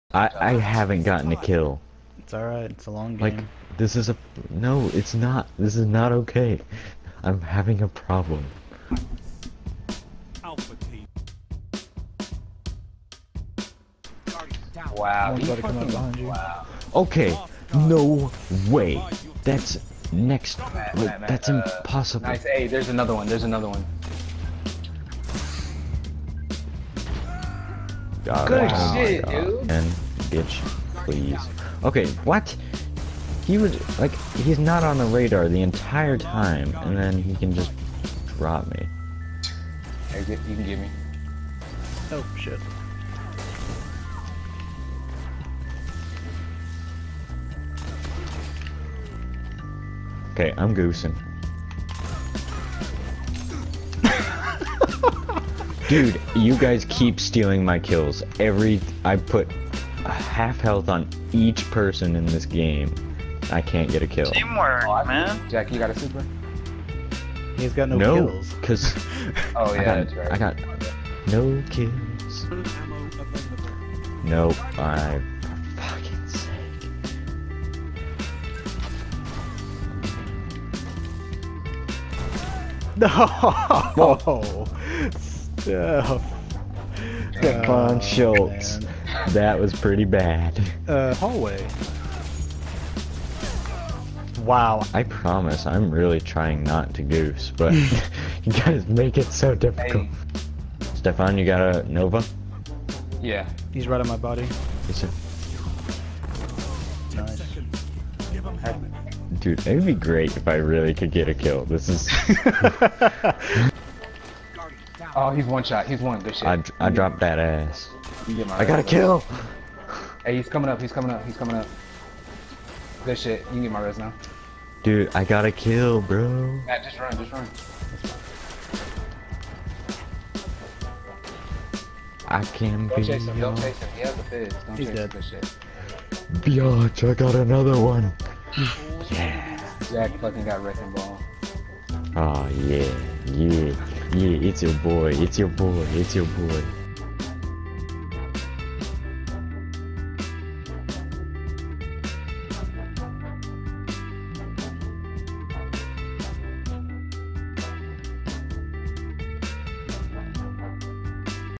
AoG Daily Short : Does Jack Goose???
Can our best PvP player really go an entire game of Trials with ZERO kills?!? Find out next time, on Dragon Ball Z.